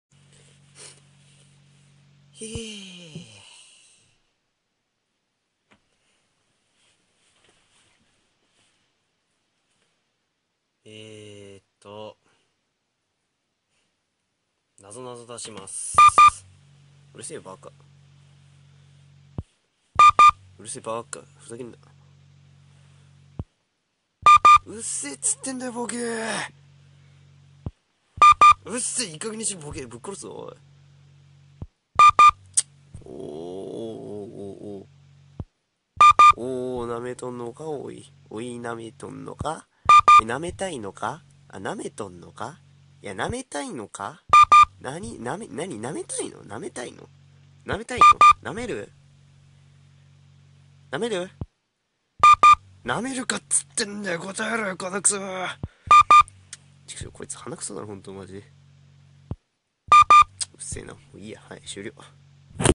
だだんの日常~げきおこ編~
なぞなぞ出そうとしたのにピーピーピーピーうっせんじゃ!